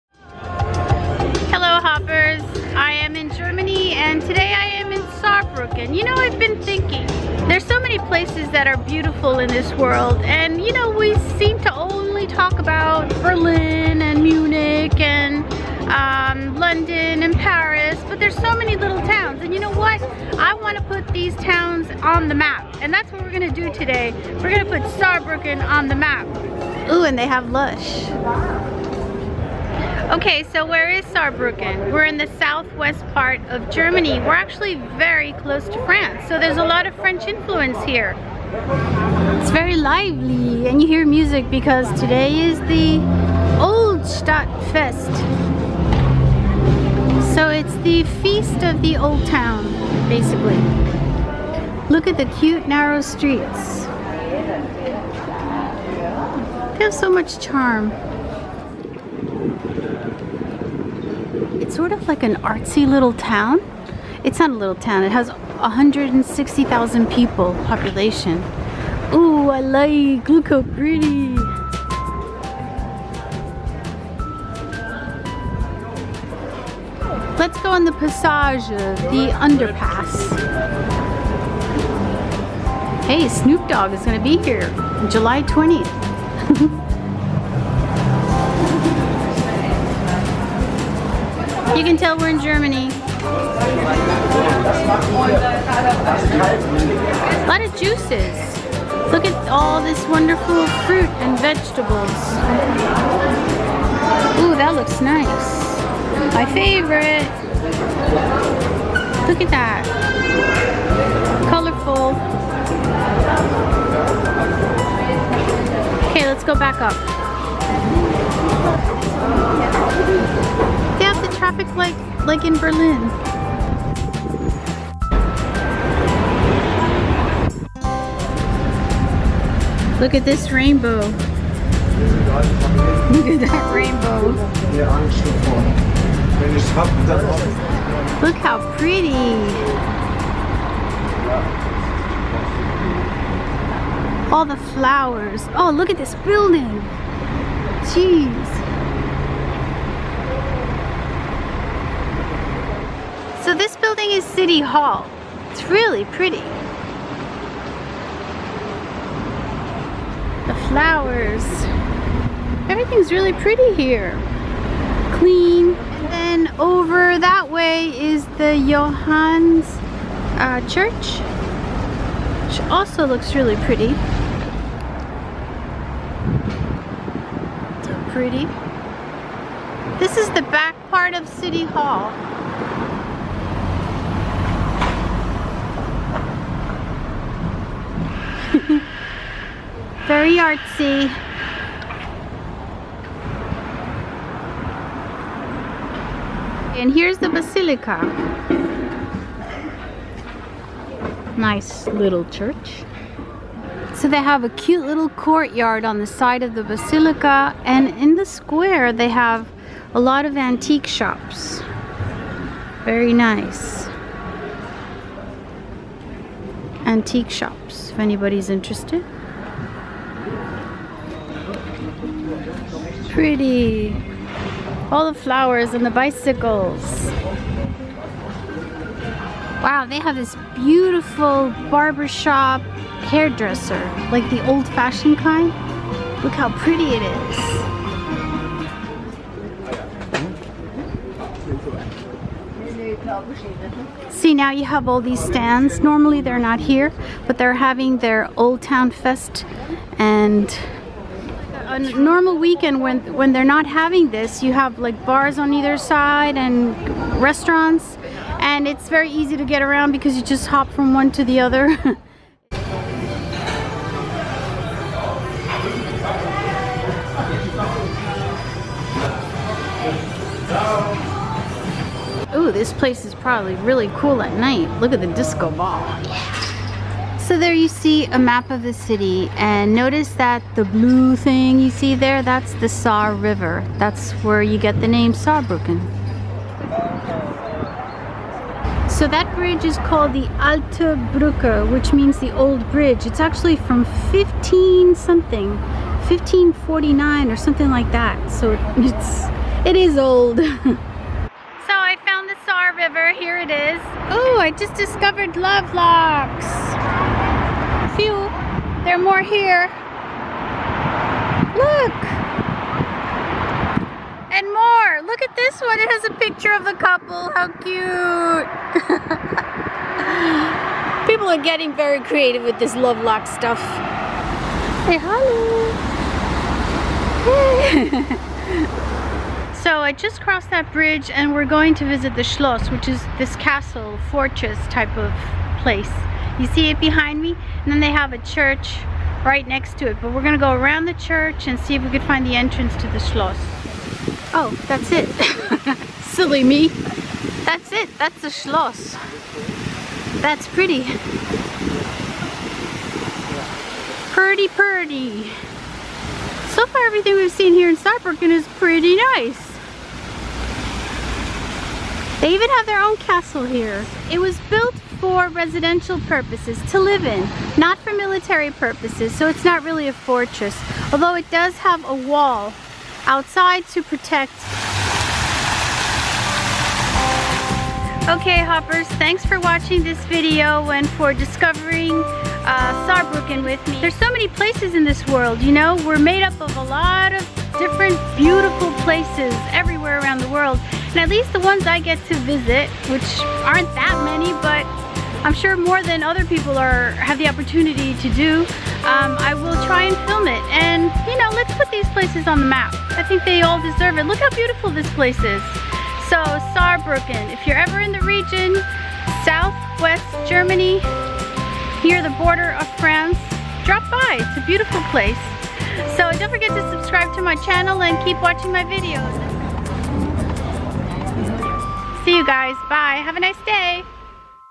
Discovering Saarbrücken - Lovely City in SW Germany
Hello Hoppers! After visiting yet another beautiful city and adding it to your list of travels, do you ever wonder how many more there are out there in the world? I often do and love to be surprised by the beauty of new place after new place. Saarbrücken is one of these places you want to come back for more. I'll be back! Hope you enjoy this video!

♡♡ I don't TELL you how to TRAVEL, I SHOW you ♡♡

Canon PowerShot G7 X Digital Camera